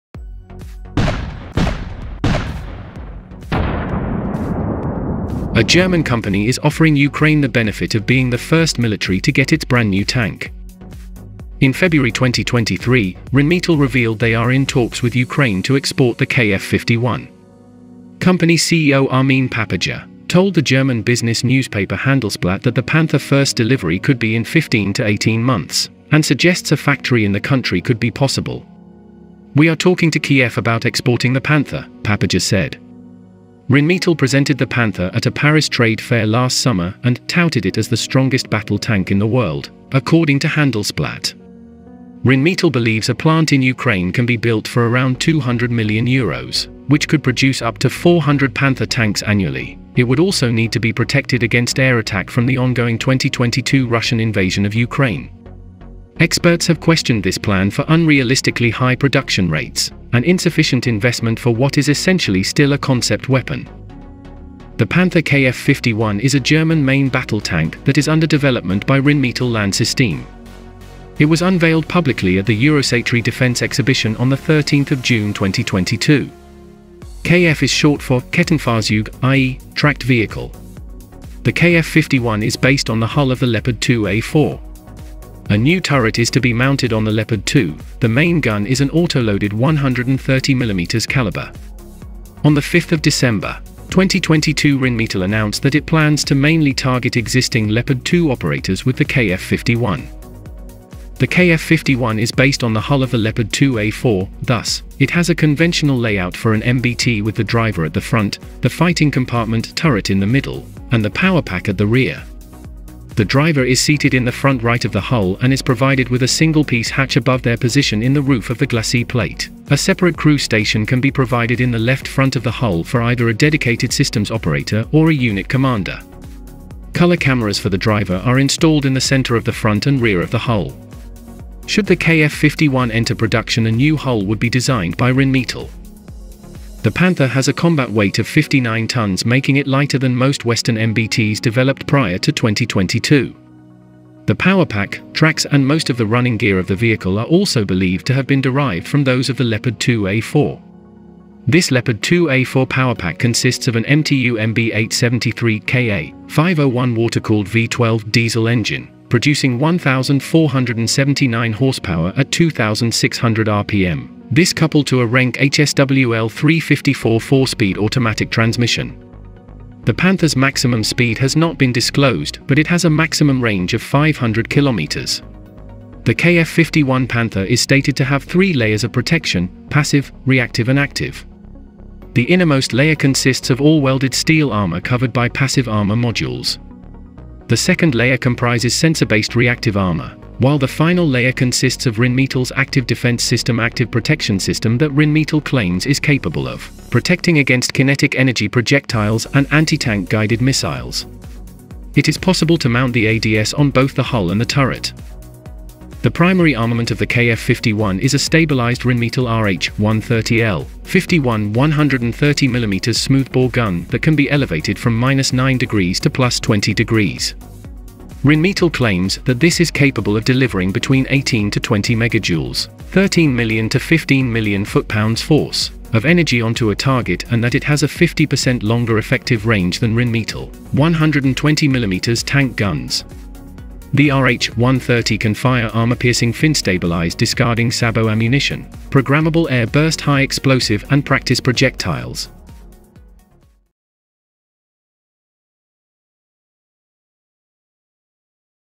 New Panther KF51 Kampfpanzer (Main Battle Tank) For Ukraine? Ukraine vs Russia Military News Updates
New Panther KF51 Kampfpanzer (Main Baytle Tank/ For Ukraine? Ukraine vs Russia Military News

A German company is offering Ukraine the benefit of being the first military to get its brand-new tank.MIn February 2023, Rheinmetall revealed they are in talks with Ukraine to export the KF51. Company CEO Armin Papperger, told the German business newspaper Handelsblatt that the Panther first delivery could be in 15 to 18 months and suggests a factory in the country could be possible. "We are talking to Kyiv about exporting the Panther," Papperger said. Rheinmetall presented the Panther at a Paris trade fair last summer and "touted it as the strongest battle tank in the world," according to Handelsblatt. Rheinmetall believes a plant in Ukraine can be built for around €200 million which could produce up to 400 Panther tanks annually; it would also need to be protected against air attack from the ongoing 2022 Russian invasion of Ukraine. Experts have questioned this plan for unrealistically high production rates and insufficient investment for what is essentially still a concept weapon.

Ukraine News. Video not independently verified
For illustrative purposes only